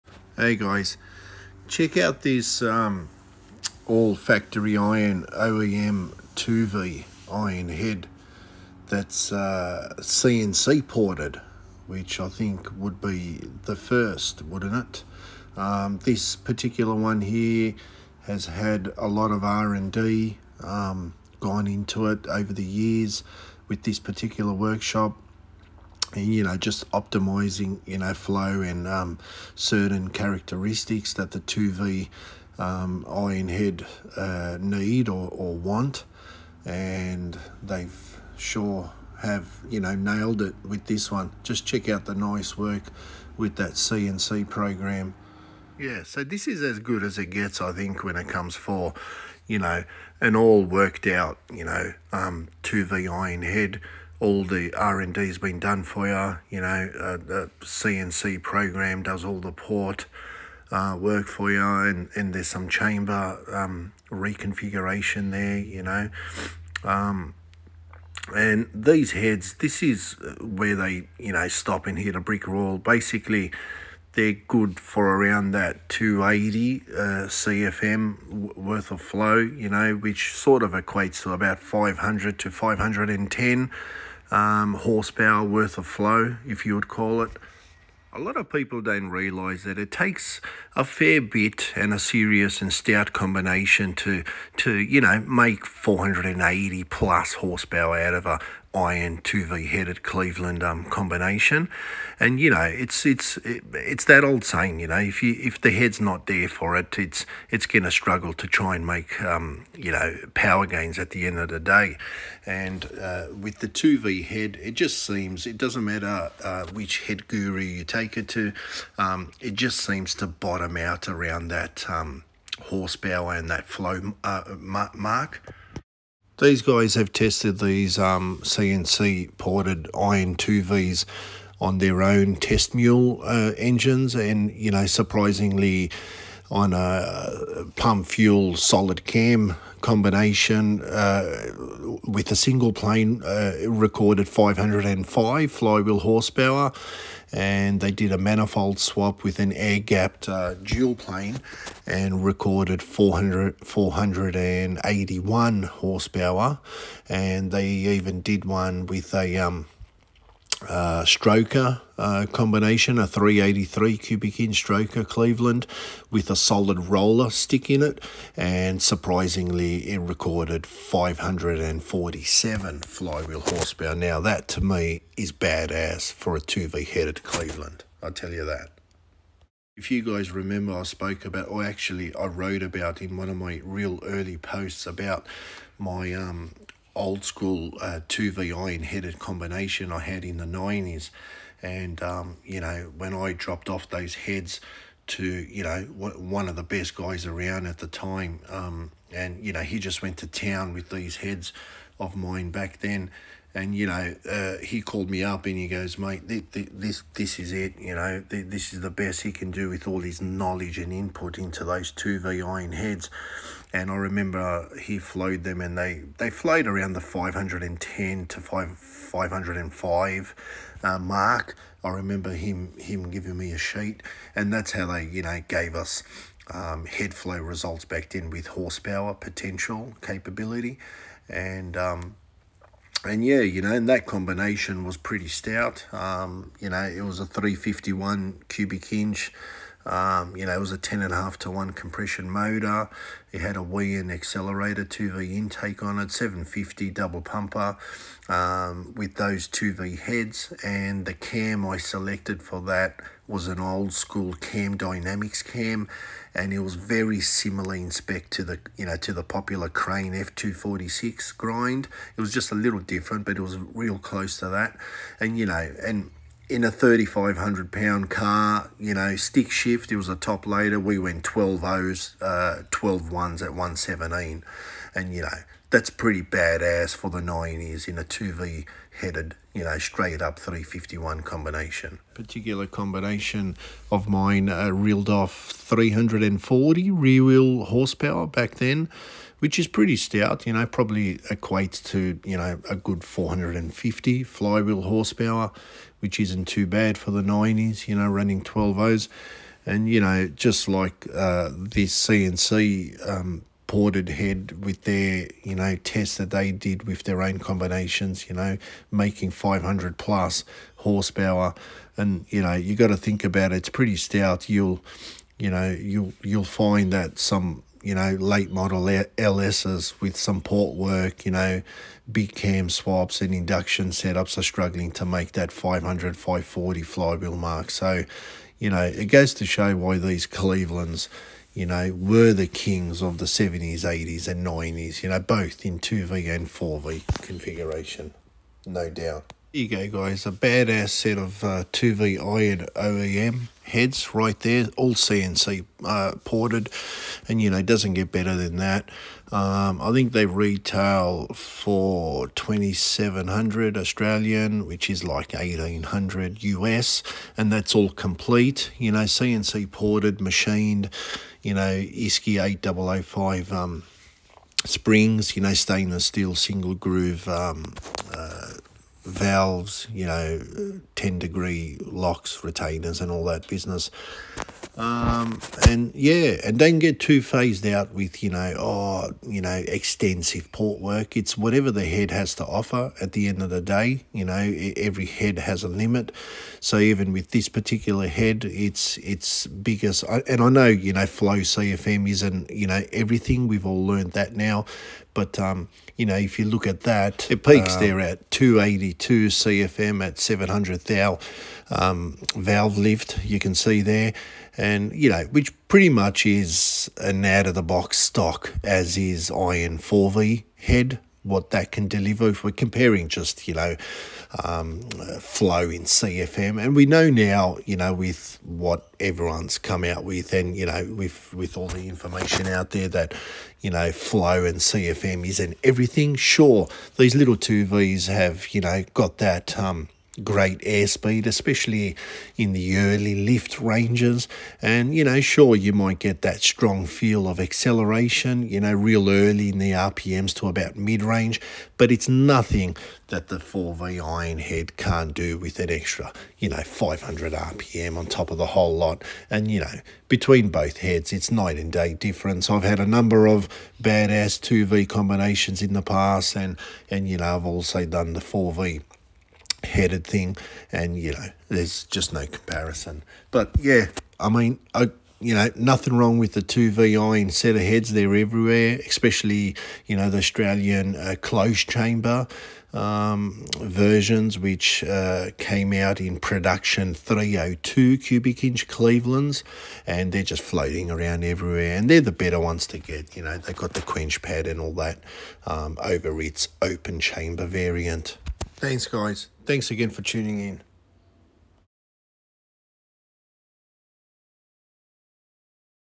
Ford 351C 2V iron heads, CNC Ported for power🏁
We take a look at these 🇦🇺 cnc ported “oem” 2V Cleveland heads & more….
(Pavtek performance 🇦🇺 vic)🏁